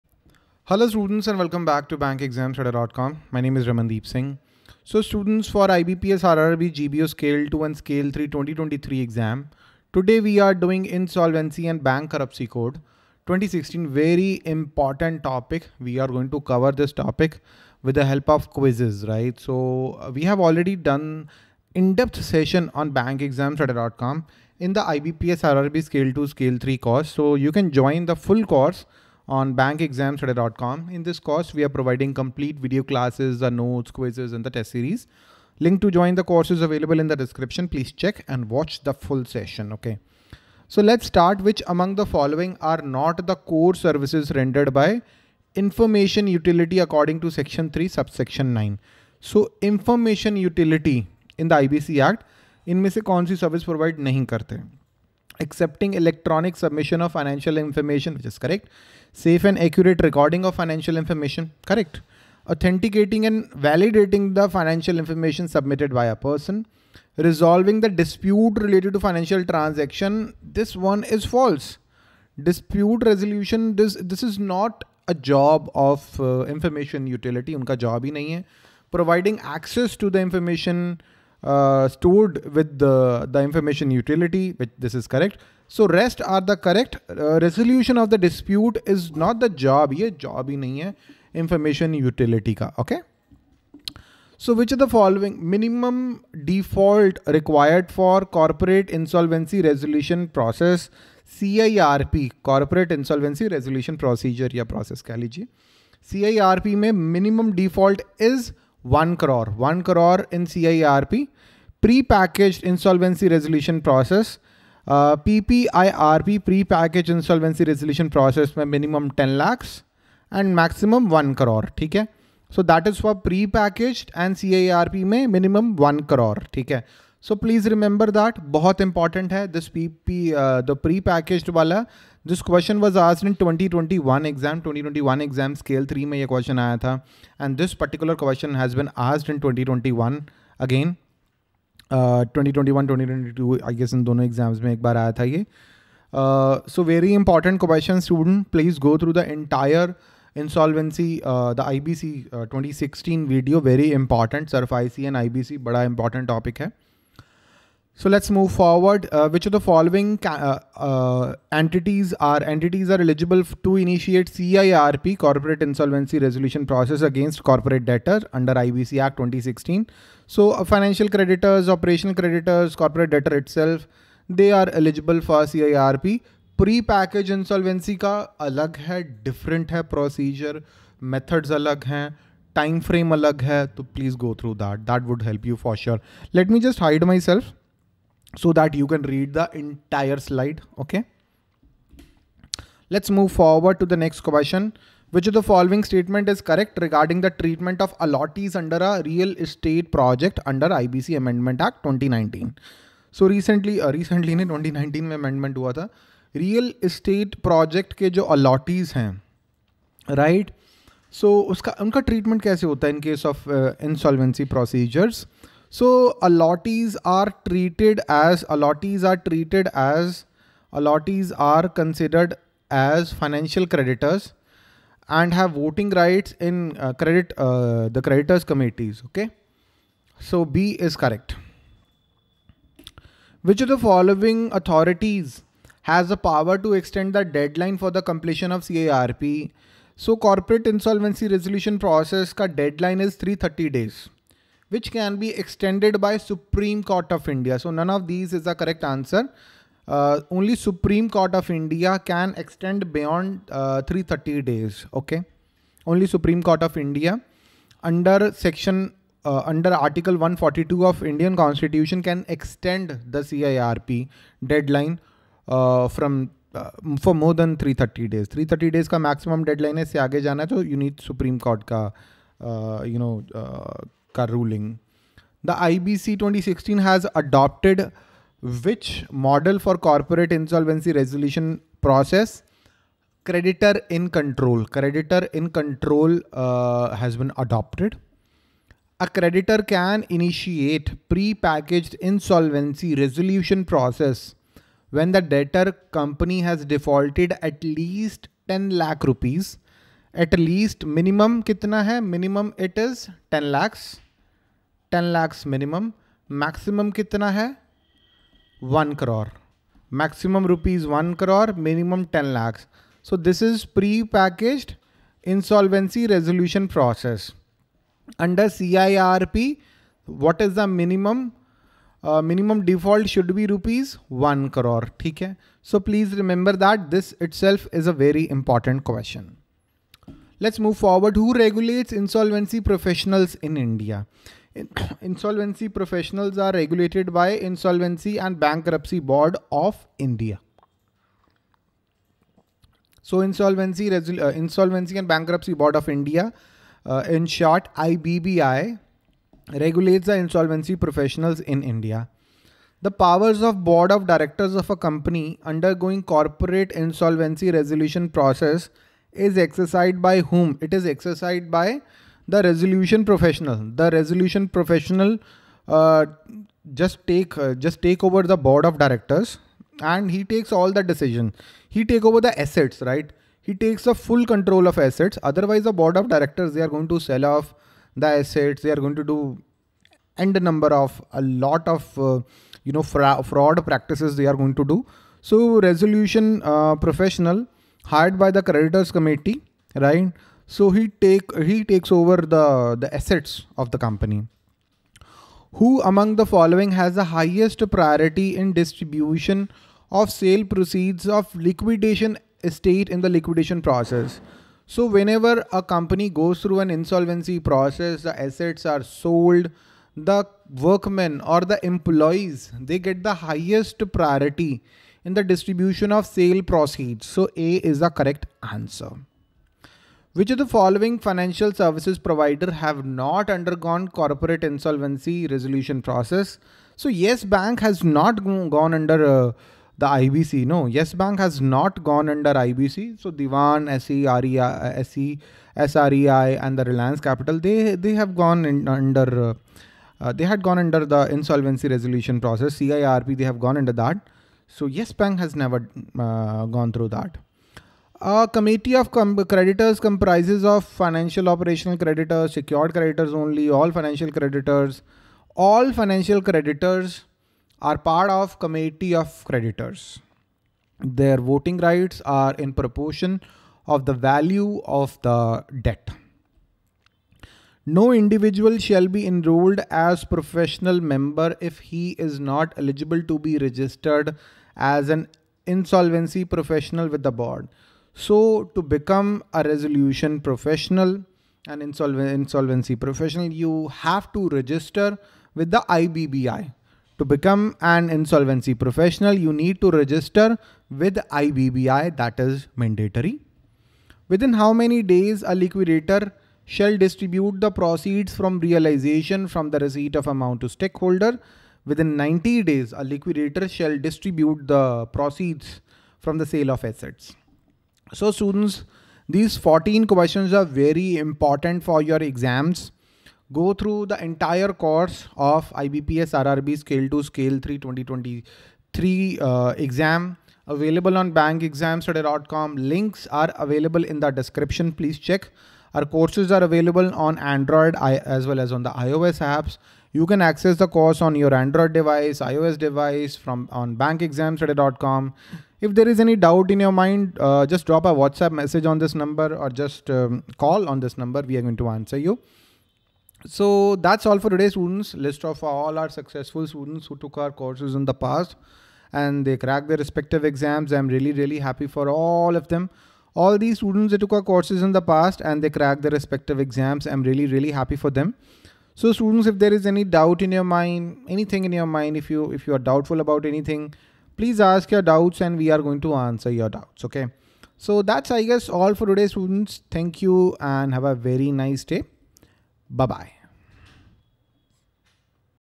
IBPS RRB GBO Scale II & III 2023: IBC 2016 Quiz
Important Financial Awareness class for IBPS RRB GBO Scale 2 and 3 (2023). In this sessions I have shared 11 important questions from Financial Instruments topic.